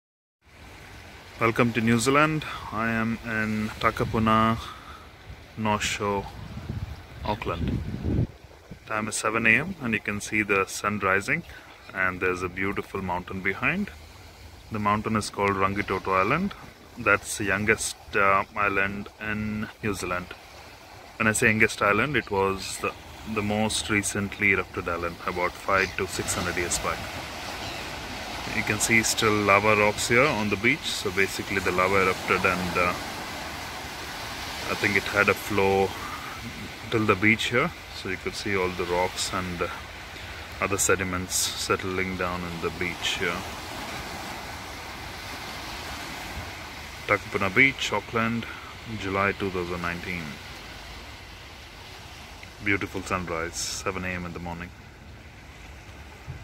New Zealand Sunrise Auckland July 2019
Beautiful sunrise with Rangitoto volcanic island in the background